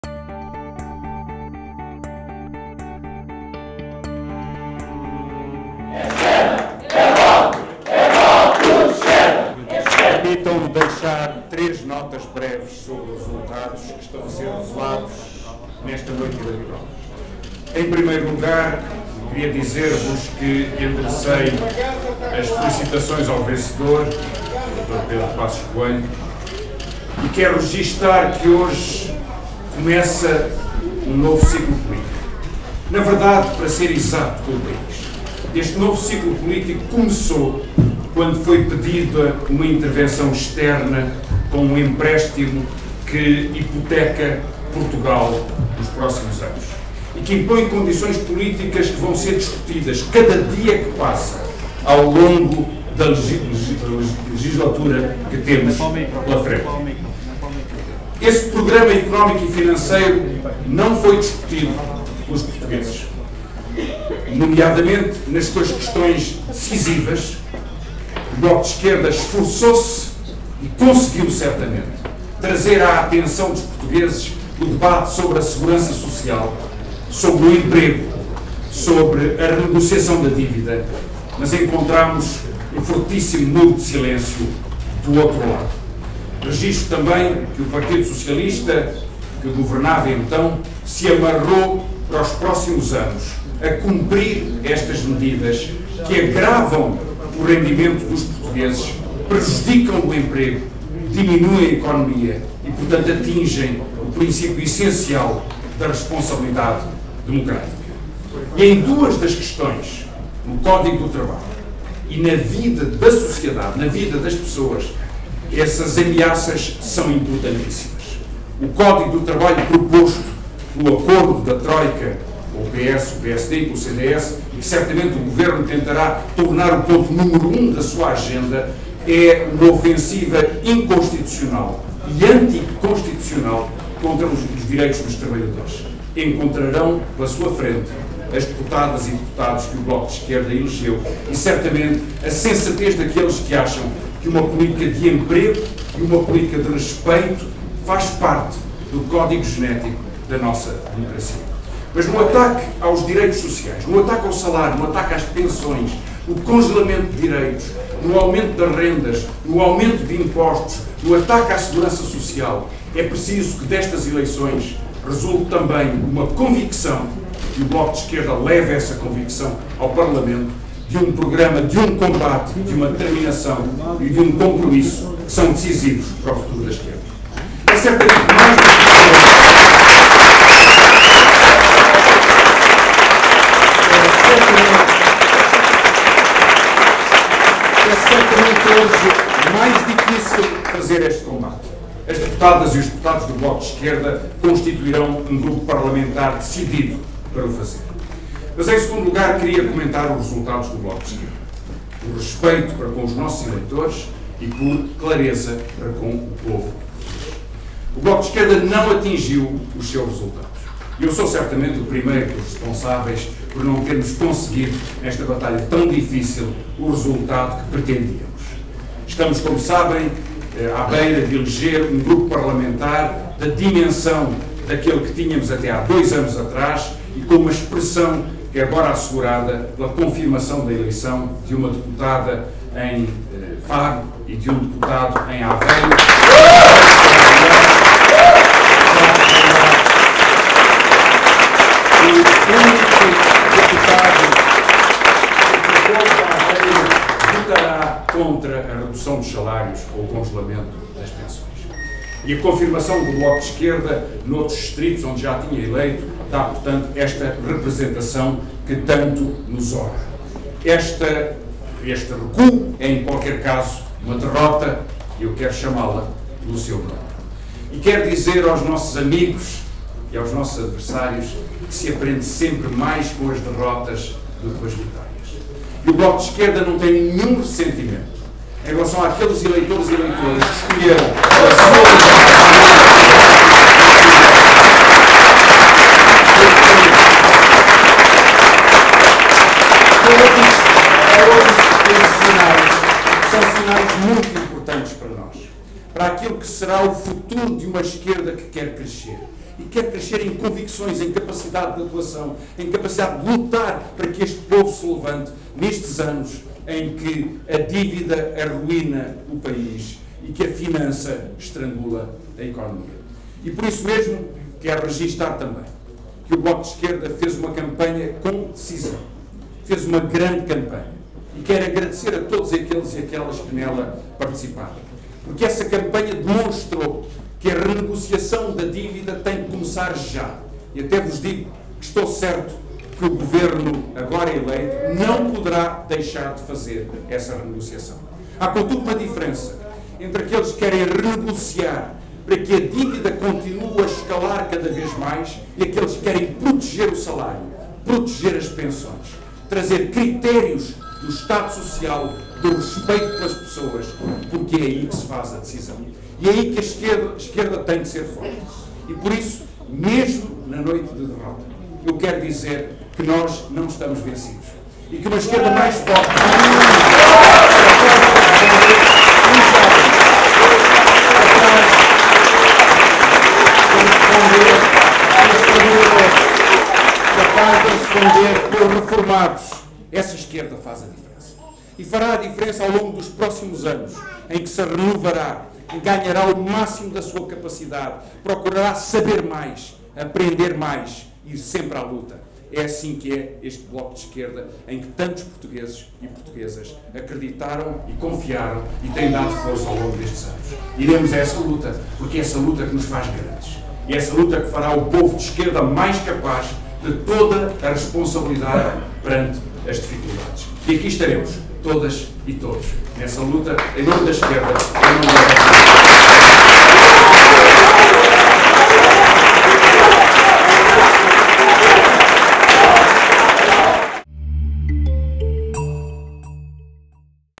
ESQUERDA.NET | "A luta é o o que nos faz fortes..."
Declaração de Francisco Louçã na noite eleitoral - Legislativas 2011